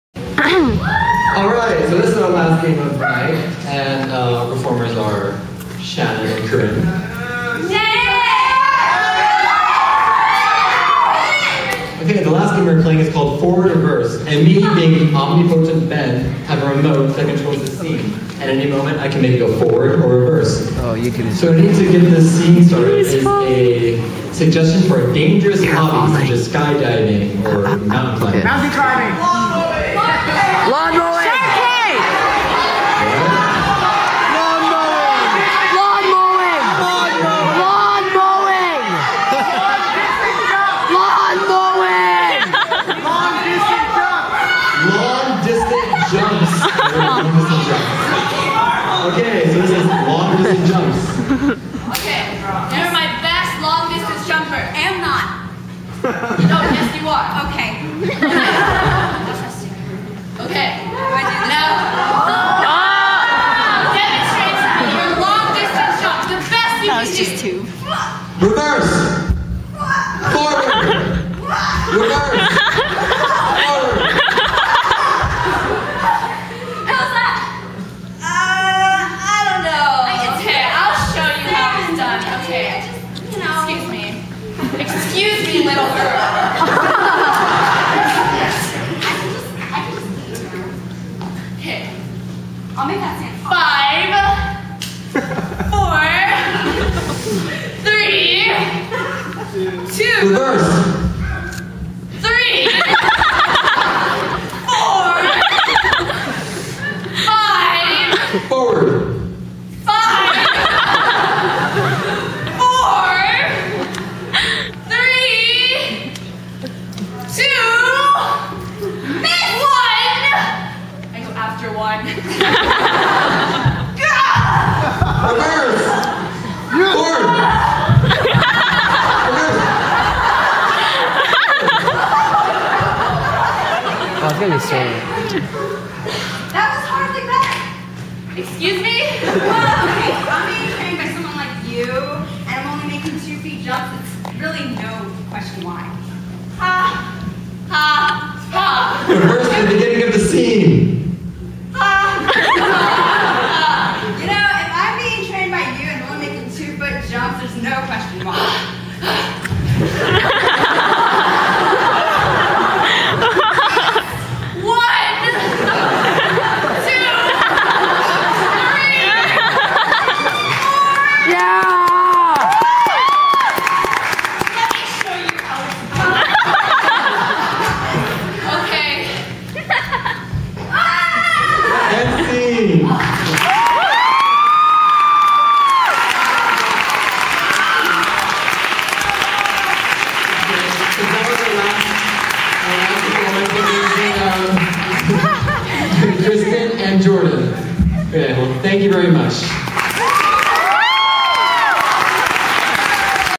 Welcome Program 2009 [Owl Tastic Improv Club Part 3]
Mid-Pacific Institute's 2009 Welcome Program. When it gets super loud, the sound kinda fails... sorry. XD